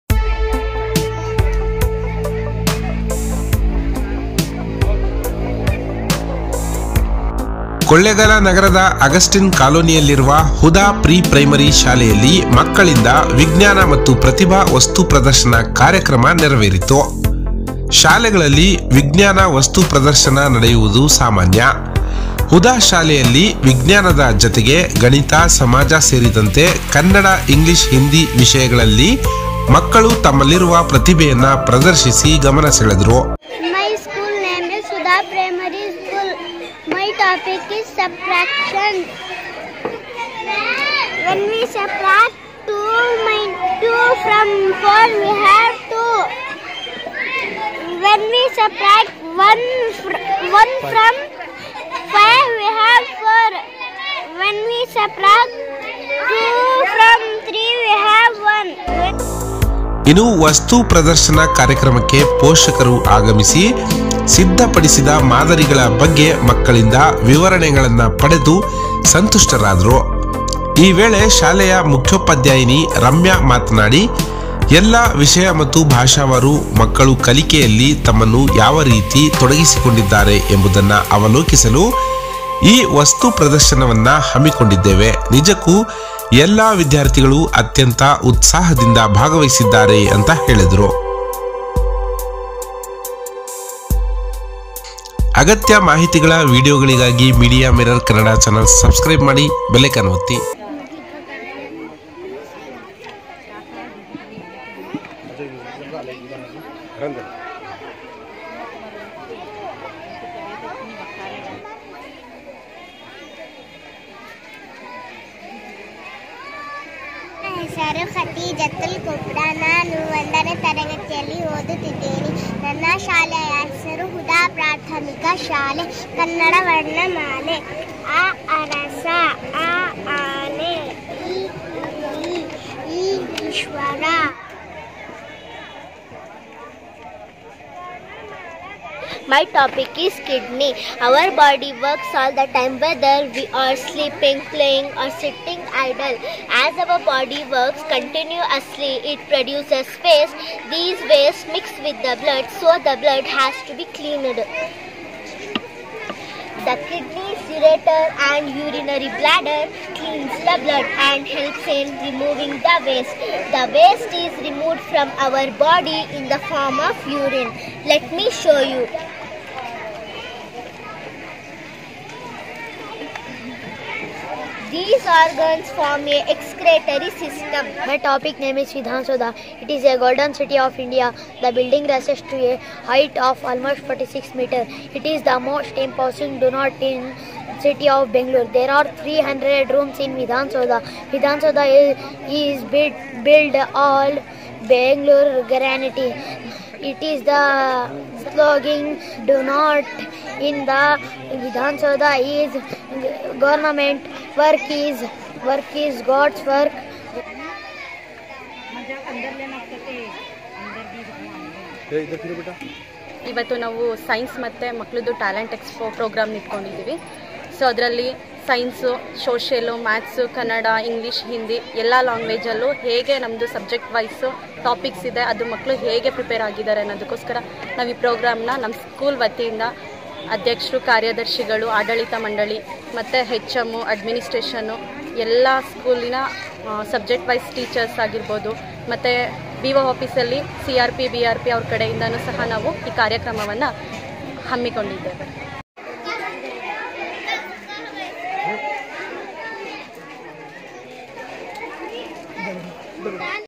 ಹುದಾ ಶಾಲಾ ಮಕ್ಕಳಿಂದ‌ ವಿಜ್ಞಾನ & ಪ್ರತಿಭಾ ವಸ್ತು ಪ್ರದರ್ಶನ
ಹುದಾ ಶಾಲೆಯ ಮಕ್ಕಳಿಂದ‌ ವಿಜ್ಞಾನ ಮತ್ತು ಪ್ರತಿಭಾ ವಸ್ತು ಪ್ರದರ್ಶನ

ಕೊಳ್ಳೇಗಾಲ : ಮಾದರಿಗಳ ಬಗ್ಗೆ ಮಕ್ಕಳಿಂದ ವಿವರಣೆ ಪಡೆದು ಸಂತುಷ್ಟರಾದ ಪೋಷಕರು